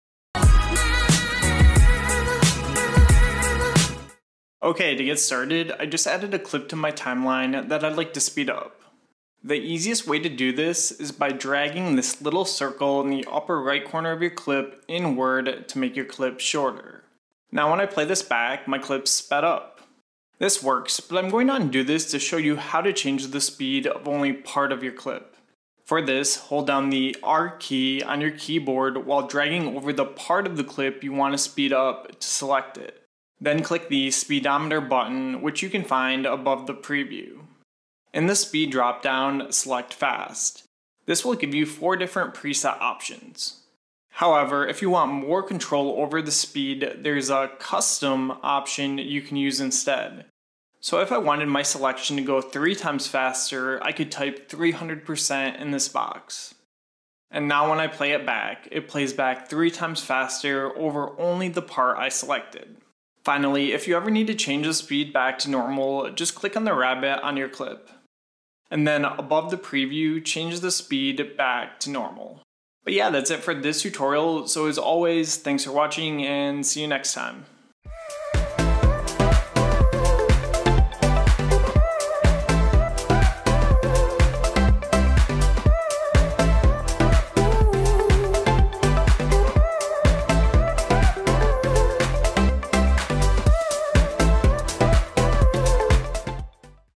iMovie : How to Speed Up Clip
A tutorial on how to increase the speed of a clip in iMovie on Mac.